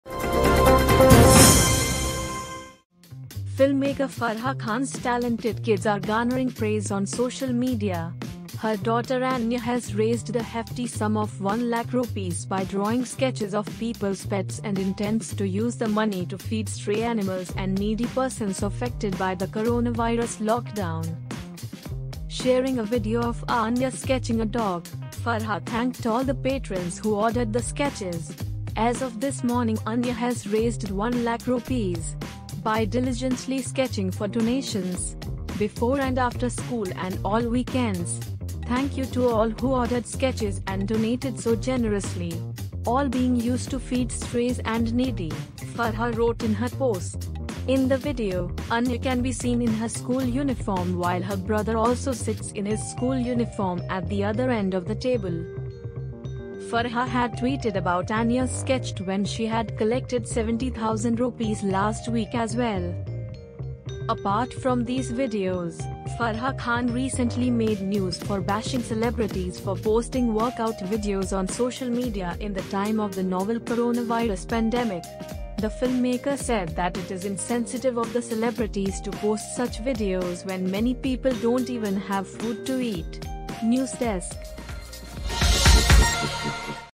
Farah Khan's daughter Anya raises Rs 1 lakh by sketching pets to feed strays and needy
Filmmaker Farah Khan’s talented kids are garnering praise on social media. Her daughter Anya has raised a hefty sum of Rs 1 lakh by drawing sketches of peoples’ pets and intends to use the money to feed stray animals and needy persons affected by the coronavirus lockdown.